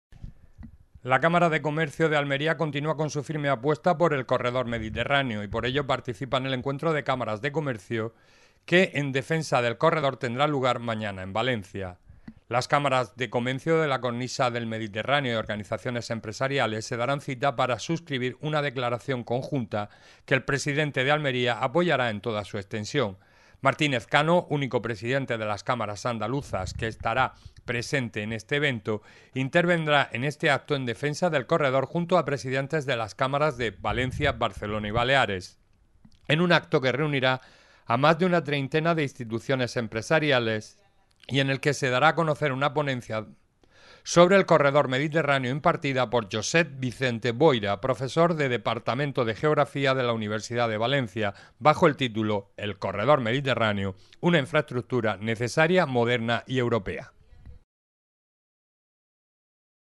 Almería Noticias Canal 28 Tv - La Cámara apuesta por el 'Corredor Mediterráneo'
La Cámara de Comercio de Almería está presente en Valencia en un encuentro en defensa del Corredor Mediterráneo.